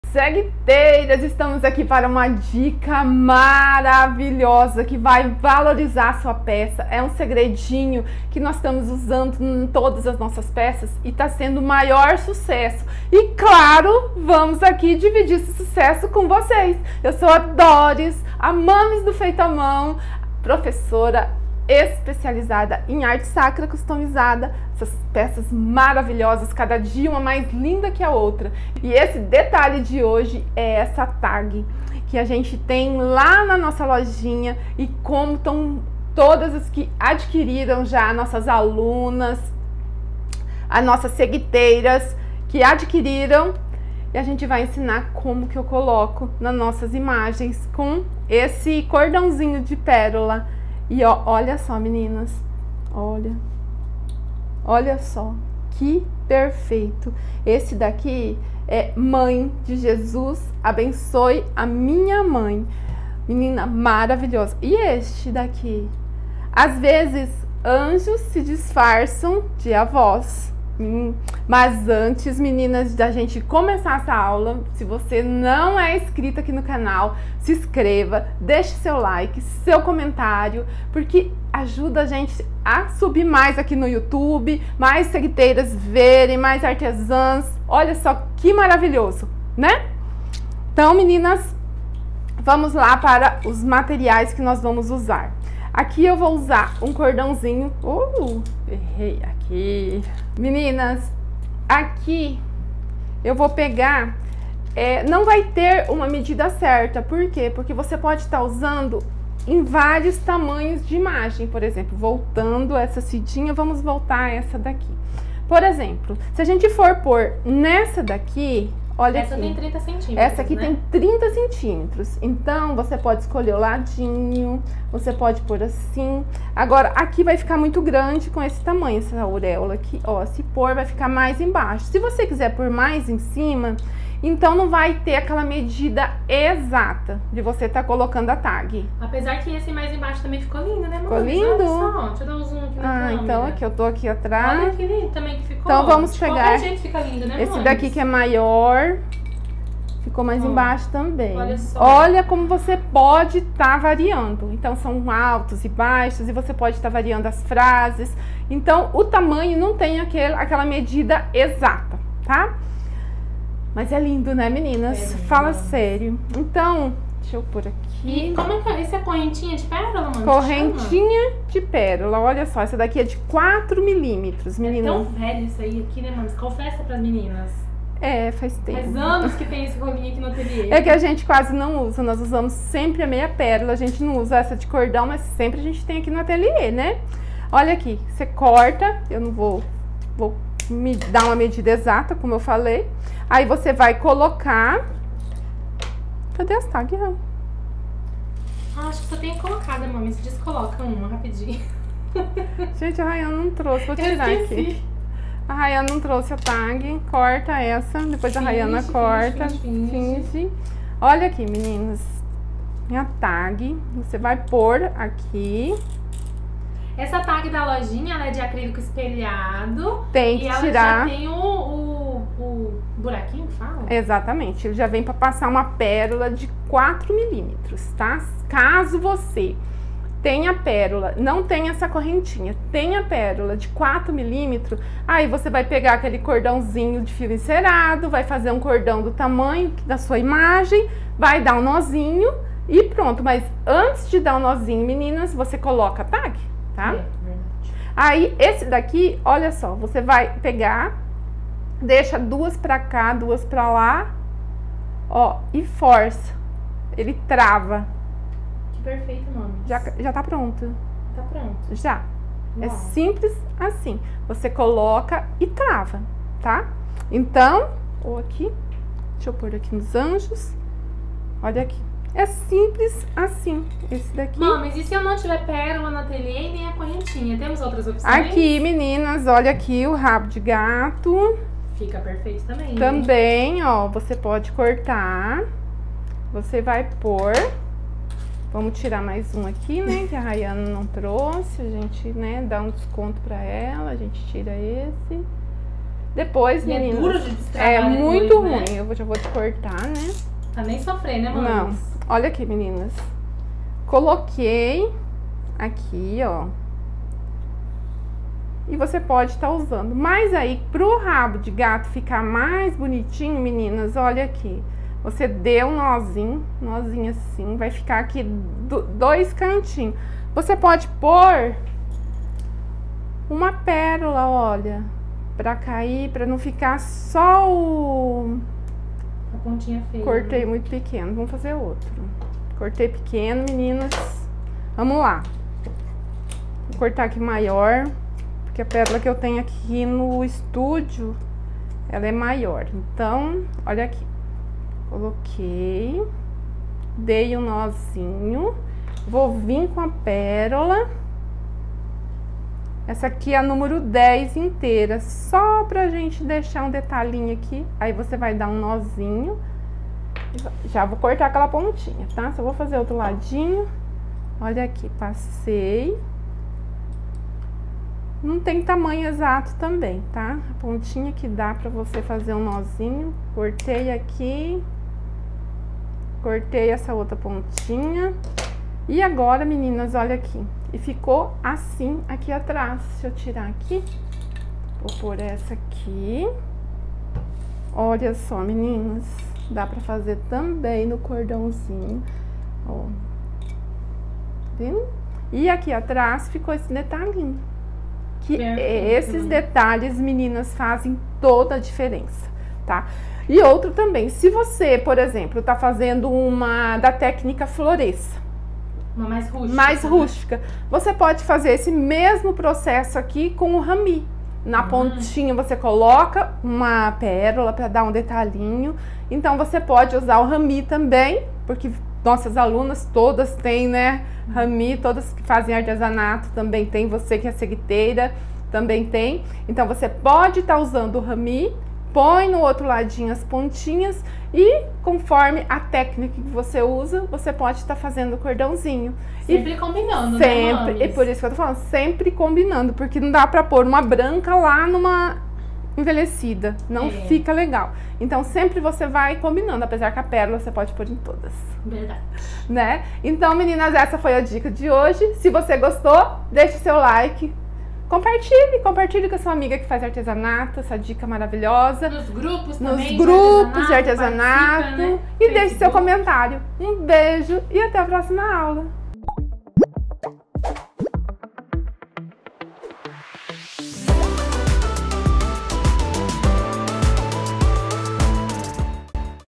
Detalhe que faz a diferença nas suas imagens!
Olá Seguiteiras de todo o Brasil e além Brasil!

Nesta aula linda de hoje vamos aprender a surpreender nossas clientes com este detalhe que faz a diferença!

MATERIAIS USADOS:

Tag que tem disponível na Lojinha FeitaMão
Cordão de perolas nº 4
cordão rami
cordão rabo de gato

Bjos até a próxima!